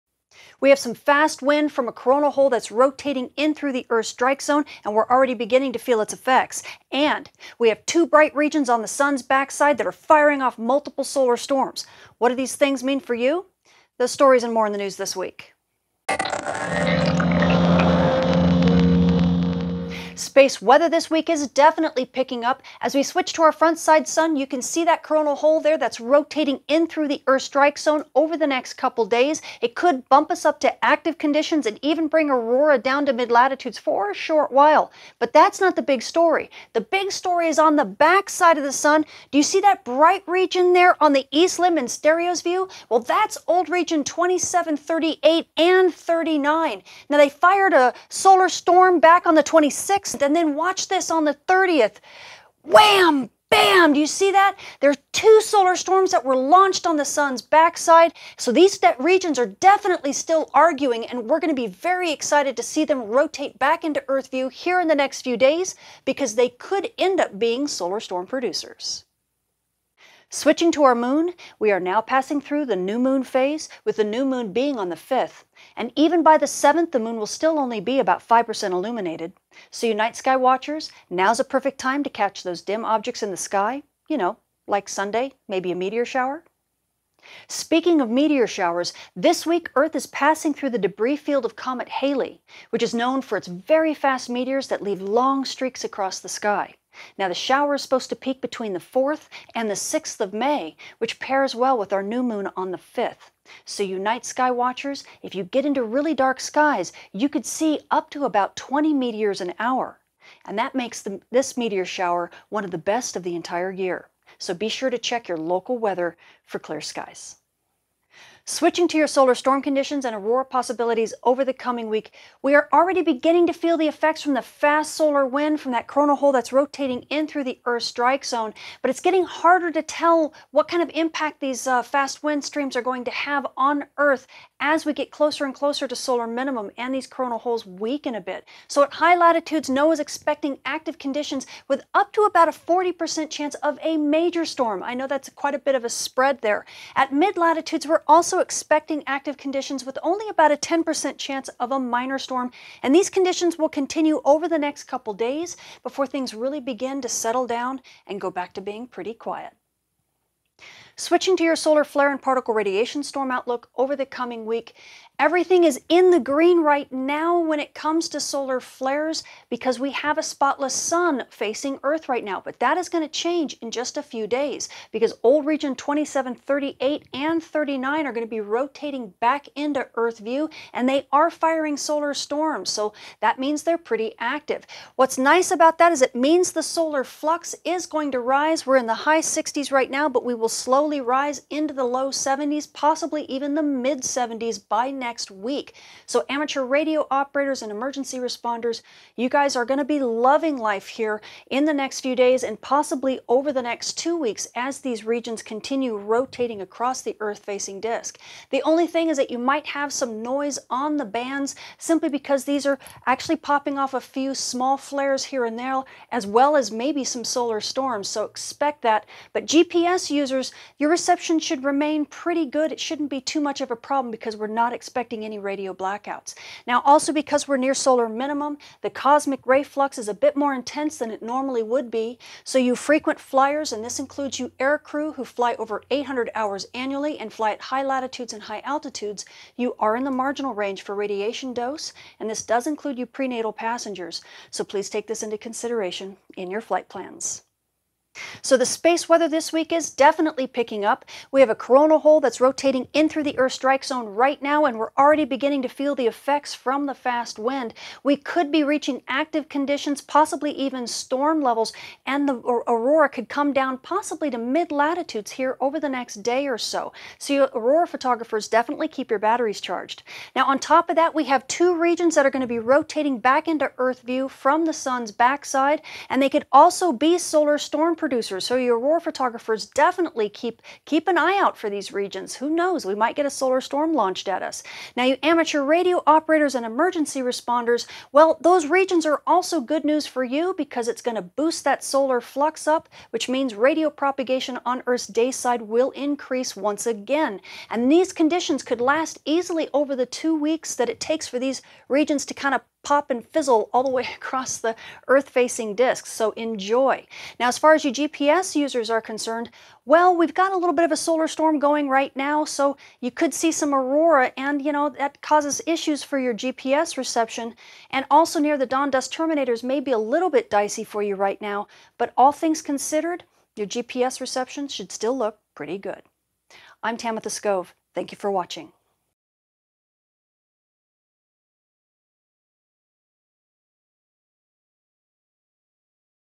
Wham! Bam! Thank You Sun | Space Weather News 05.02.2019
Our Sun really wakes up this week as we not only have some fast solar wind hitting Earth now that has already brought us up to active conditions, but we also have at least 4 solar storms that were launched on the Sun's backside in less than 48 hours! Old regions 2738 and 2739 seem to be in an active region shootout to see which one can launch the biggest solar storm-- Wham! Bam! They are firing fast! As these regions rotate back into Earth view over the next few days, we will get a better look at them to see if they could possibly launch an Earth-directed solar storm once they reach the Earth-strike zone. Meanwhile, solar flux increases slowly, but as these regions rotate more into view, we could see flux rise back into the mid-70s. This means radio propagation could improve to marginal levels by week's end! Learn the details of the fast solar wind hitting now, how long aurora might last, and what these returning regions have in store for us!